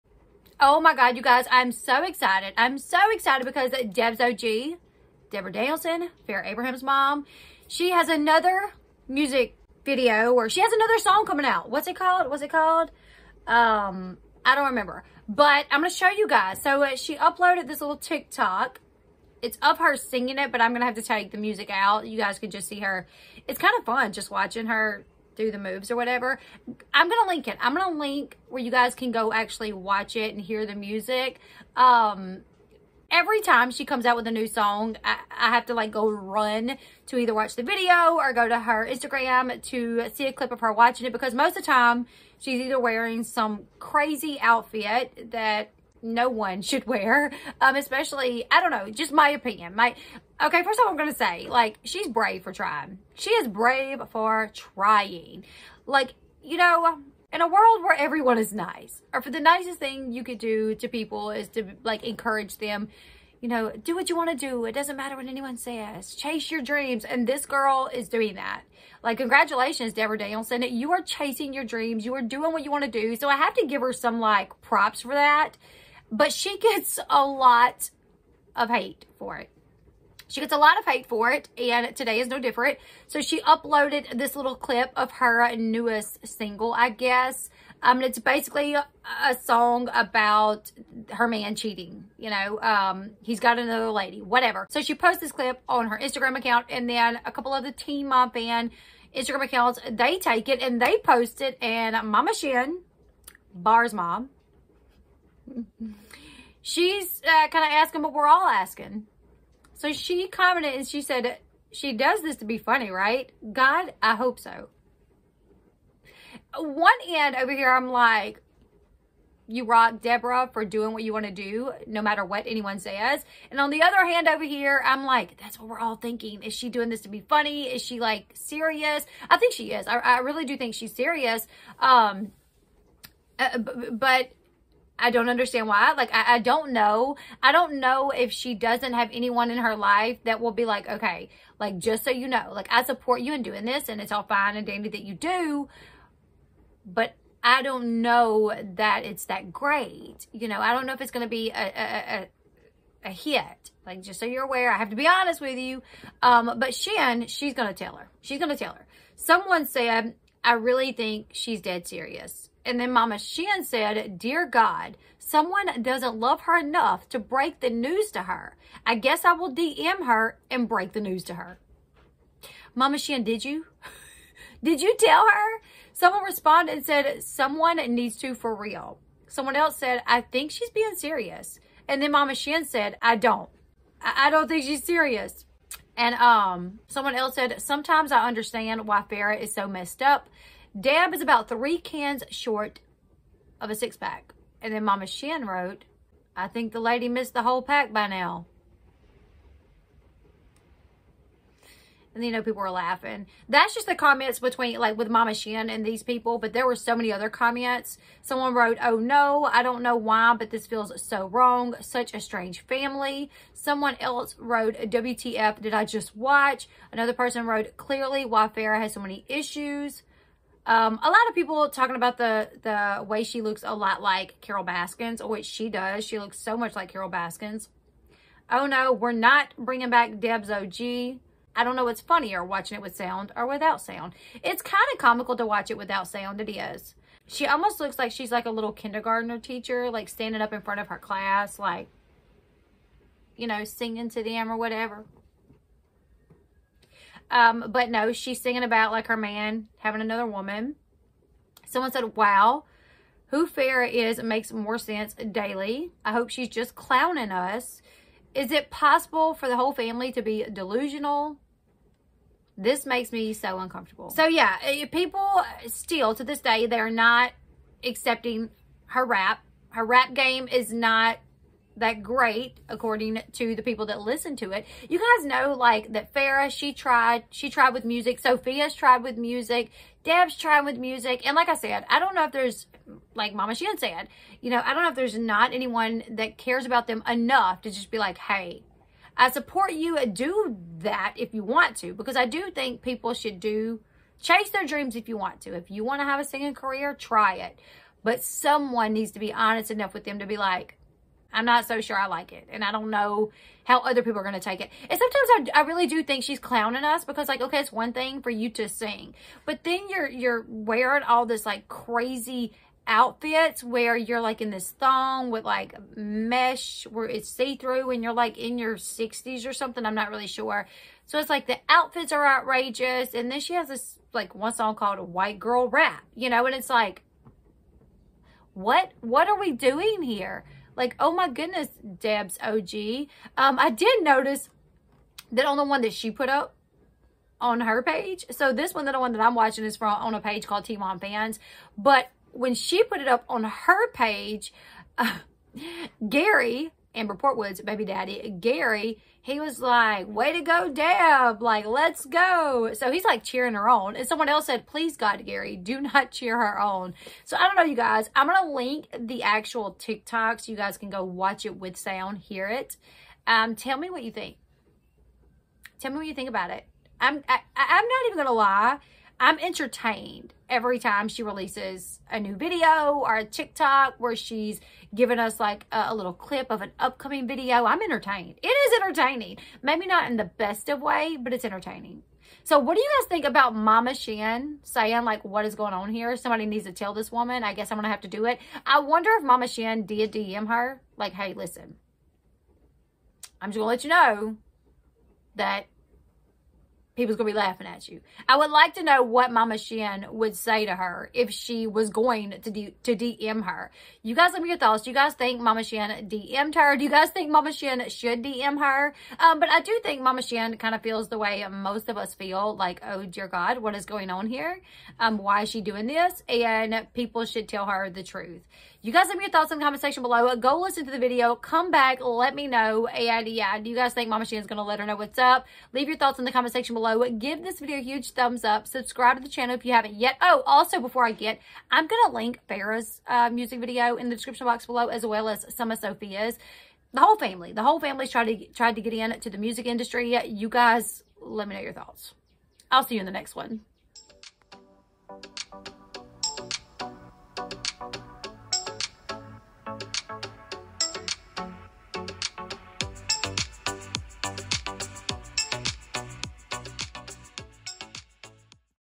MamaShen Ready To Tell Debz OG How She Feels About Her Music! Deb Releasing New Song! Shen Not A Fan
Check out The Pour House Website to find your new favorite candles!!

$EllBee7

Source of information:
Teenmomfanz IG
Momma Shen IG
Debra Danielson IG

Send Friend Mail to:
Elle Bee
Florien, LA 71429